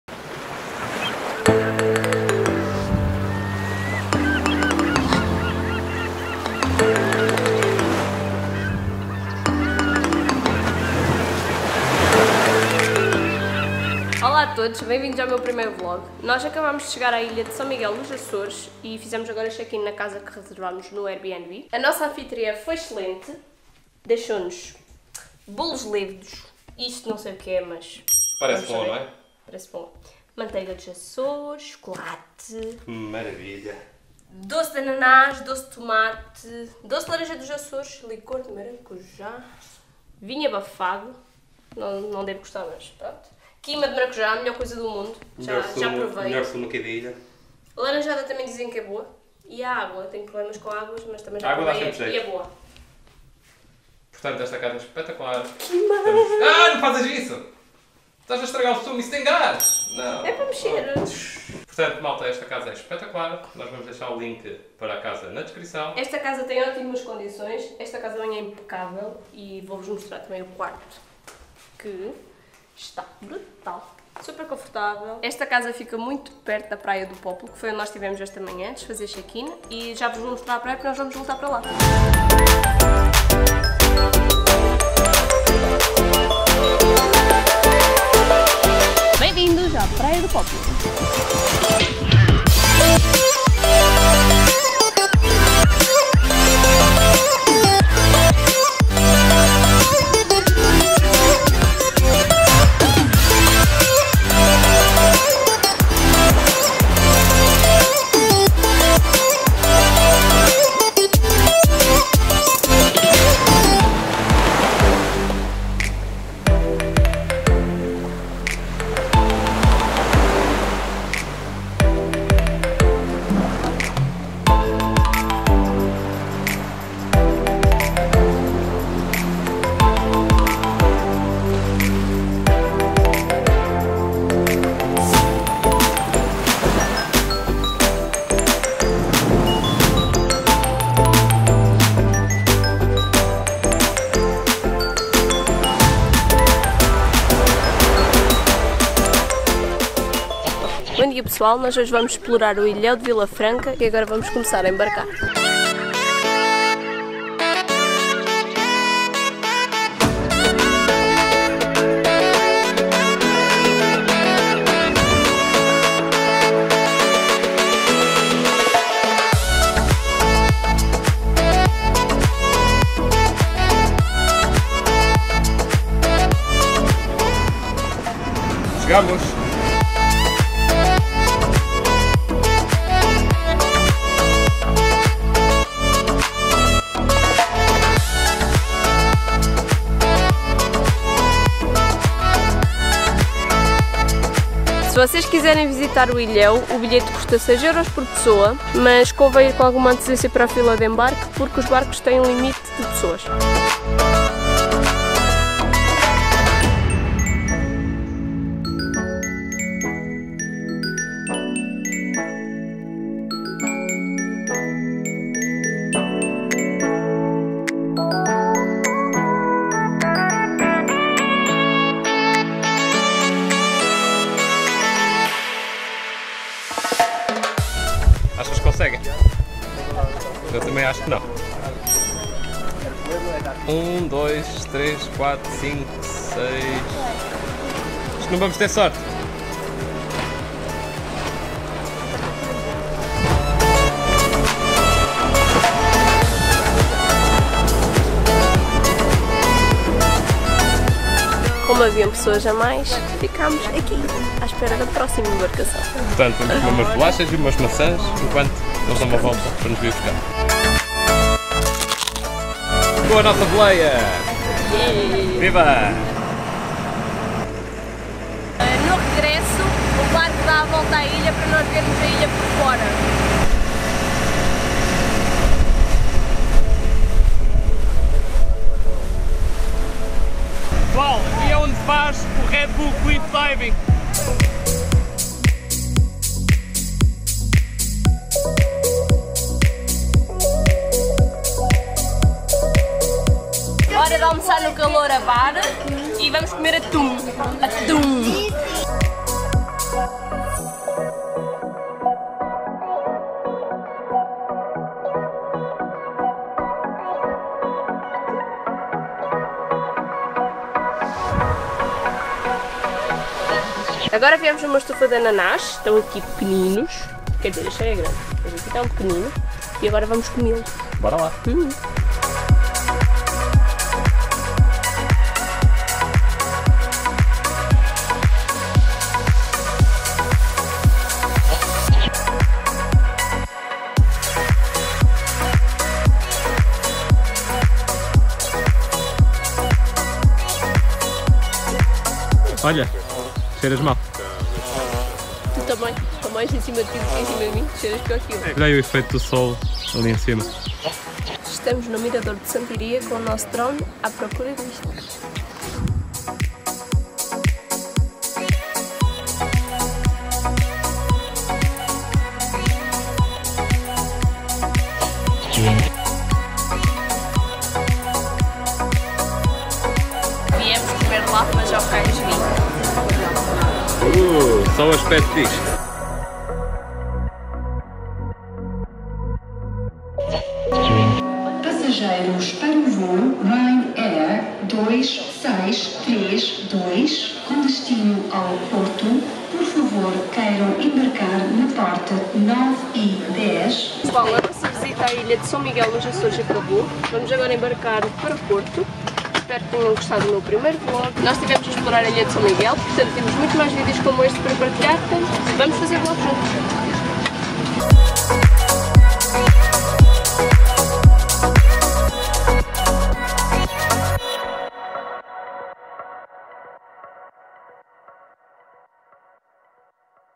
Viagem à Ilha de São Miguel
Vlog da nossa viagem à Ilha de São Miguel nos Açores. Ficámos alojados perto da Praia do Pópulo, fomos visitar o Ilhéu de Vila Franca, fomos conhecer a Fábrica de Chá Gorreana, visitámos o Miradouro de Santa Iría, fomos ao Cais 20 Restaurante Marisqueira-Bar comer as famosas lapas grelhadas dos açores, mas visitámos muitos mais locais, se quiserem ver quais foram sigam-nos nas nossas redes sociais! A introdução foi gravada na Praia da Ribeira Quente, que foi uma das praias que mais gostámos.